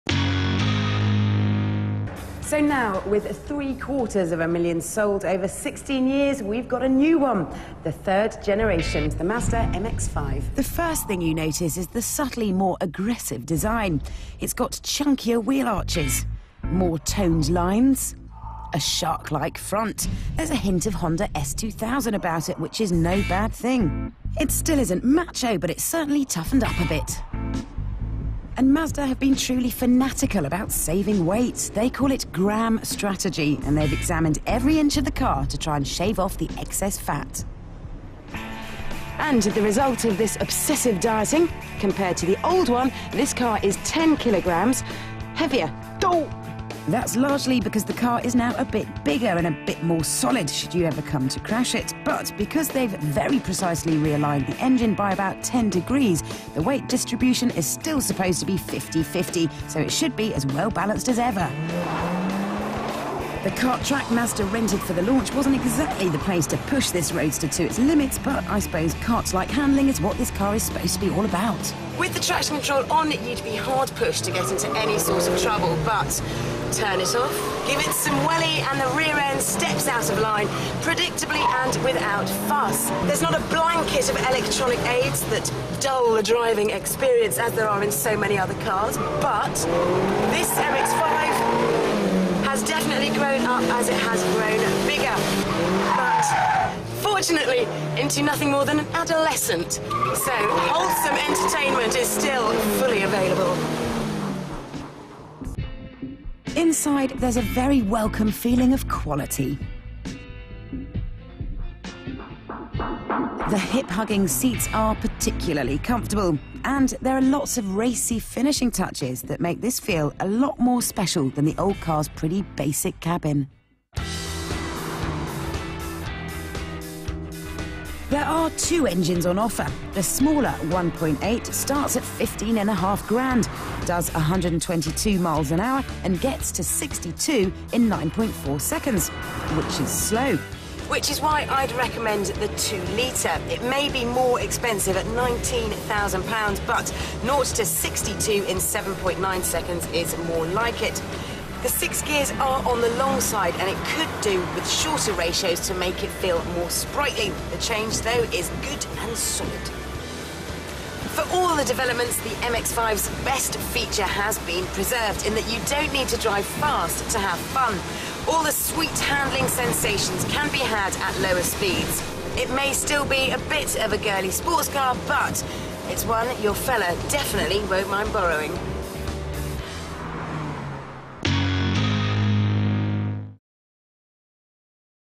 Mazda MX5 - Road Tests
The unusual door arrangement and radical rotary engine make this car stand out but will it impress Tiff?